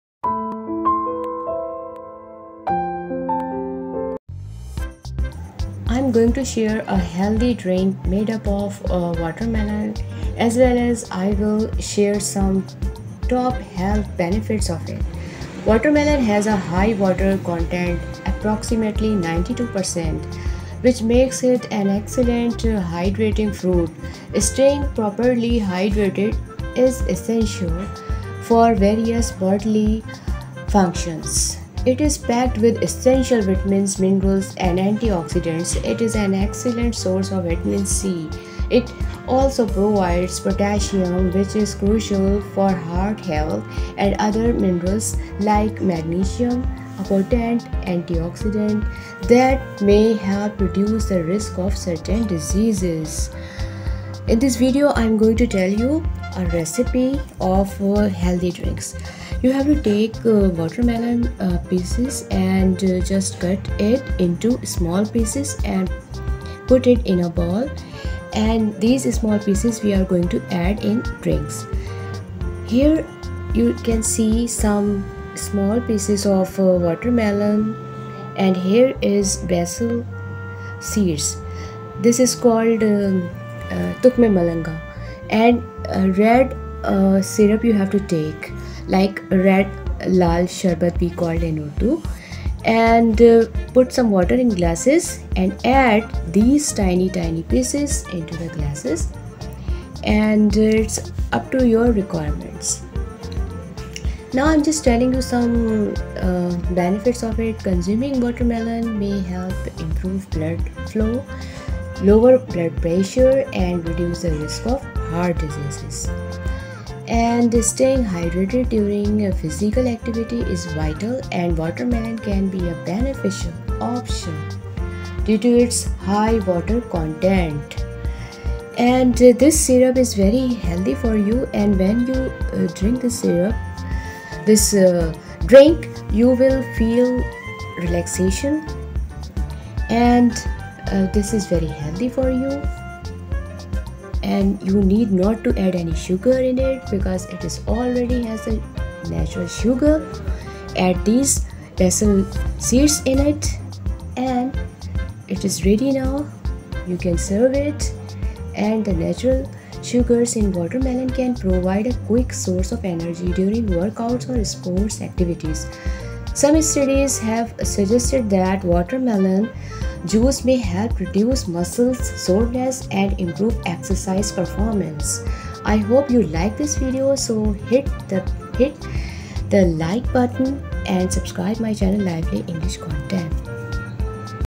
Watermelon health benefits | Watermelon juice recipe | Tarbooz kasharbat
This video is about the health benefits of watermelon and its juice.